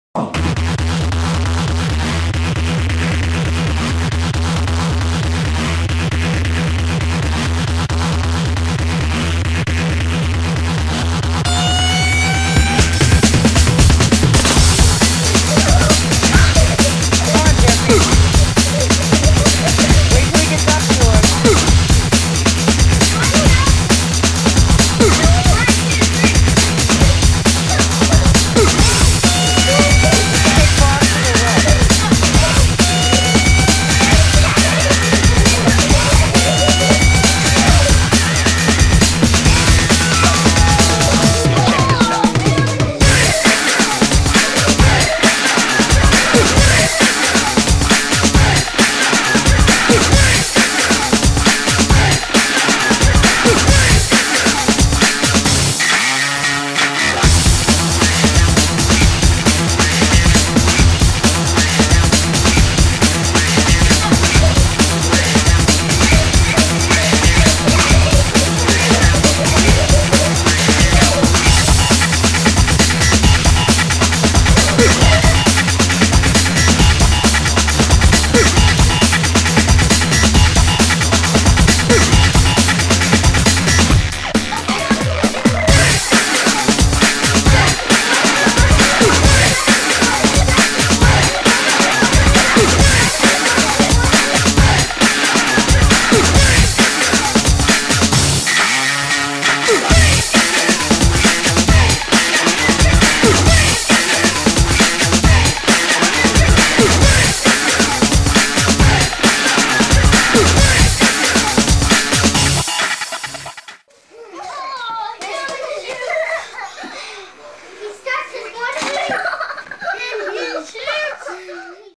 TKO Little Bro
I was looking through old videos and I found me and my brother boxing when we were younger.  It was hilarious because he hits me in the lip and I knock him out...so I had a little fun with it and put some music in the background.